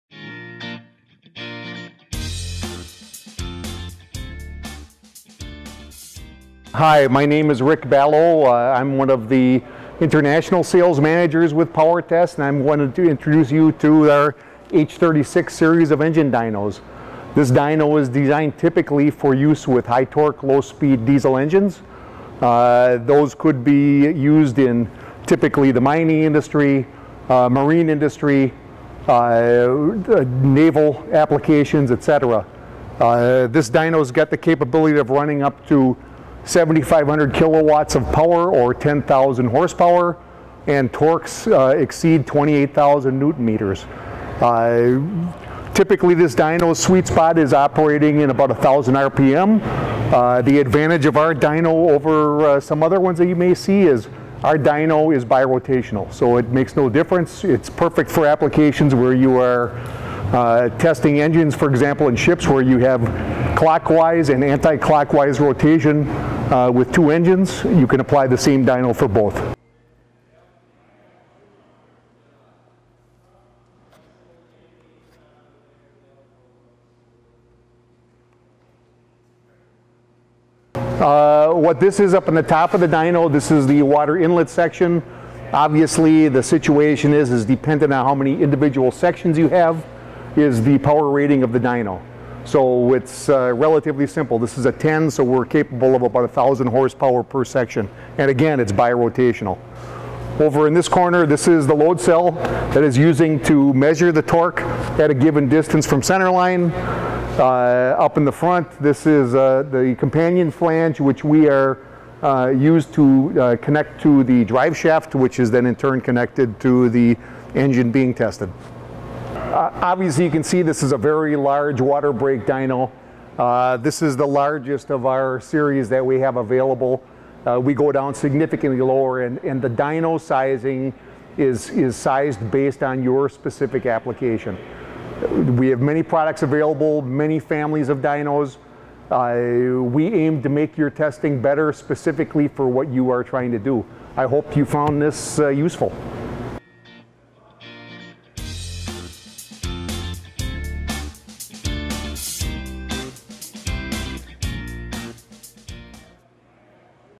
H36 Series Water Brake Engine Dyno
The Power Test H36-Series diesel dynamometers are high torque, low speed diesel engine dynamometers. Built with maximum productivity in mind, they can be found servicing mining, marine, construction, power generation and marine propulsion industries around the world. Choose from six different models with application power ranges up to 10,000 HP (7,455 Kw). All products are backed by unsurpassed support. Our experts travel the globe to work with you, anywhere and anytime.